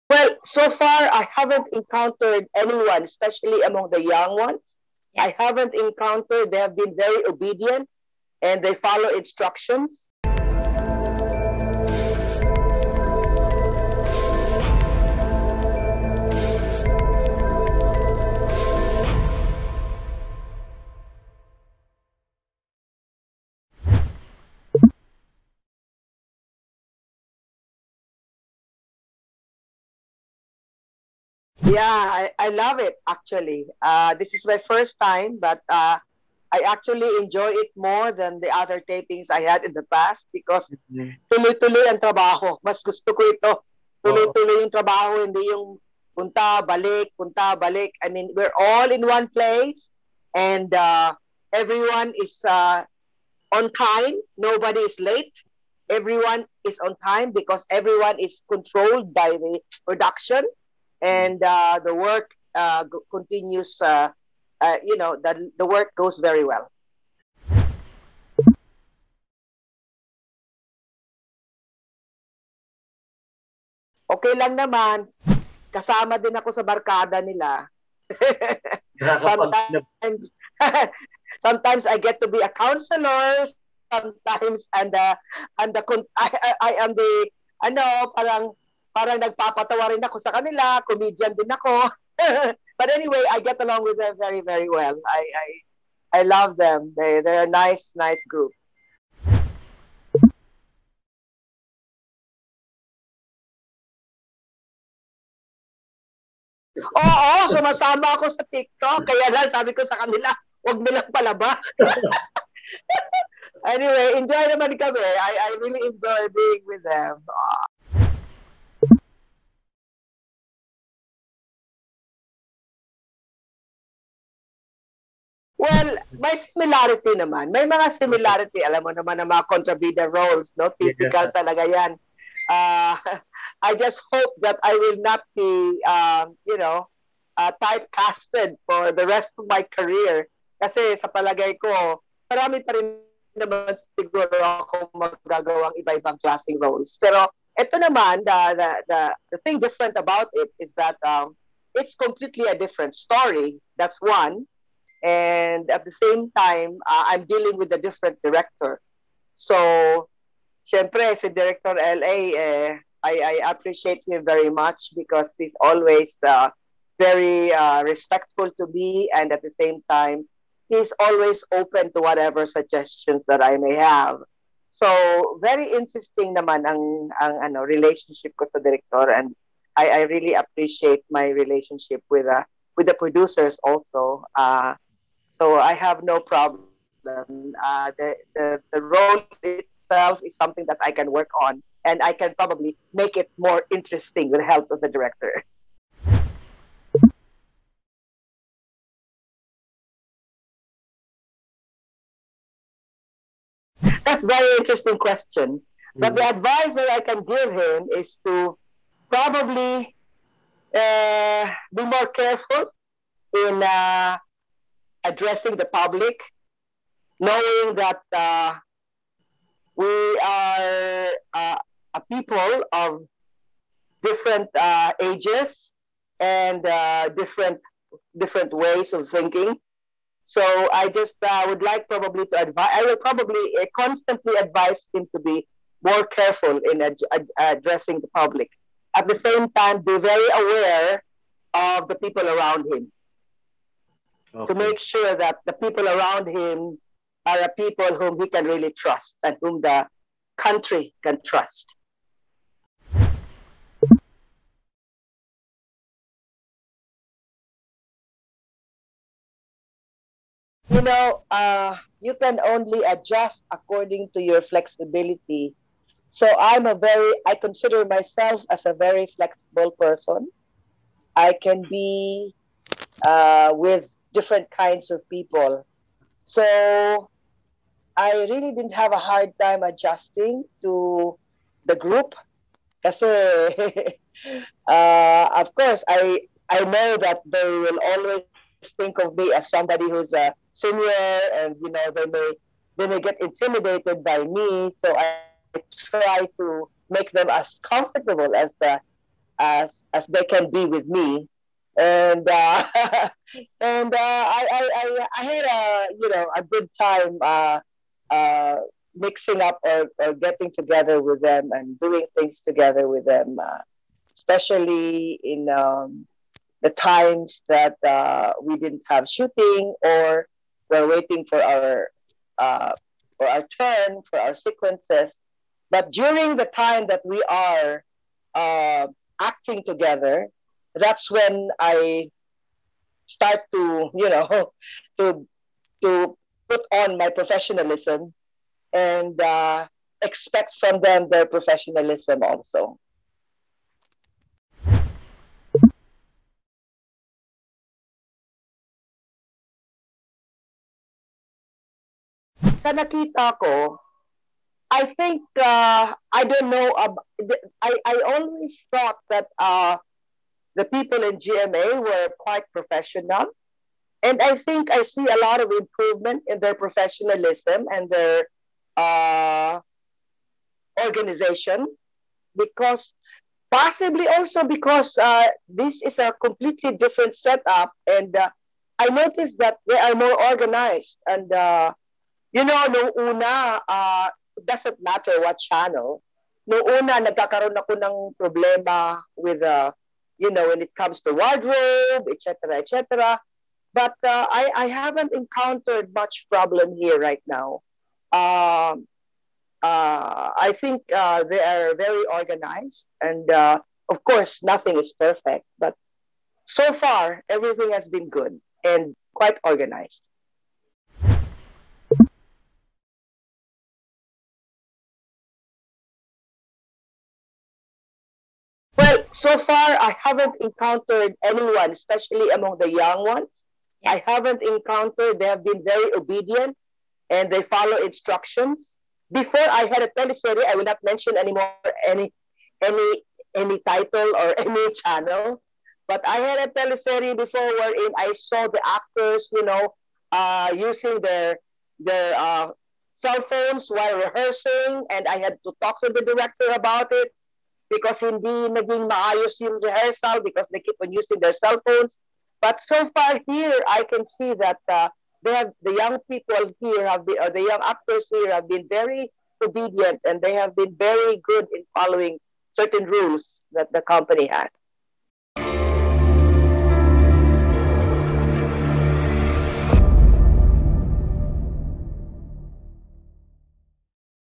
Pilar Pilapil gives Pres. Rodrigo Duterte some advice
Veteran actress and former beauty queen Pilar Pilapil shared some advice for President Rodrigo Duterte. During the virtual media-con of GMA Network's First Yaya, she was asked if she happened to be the president's mother, what advice can she tell him?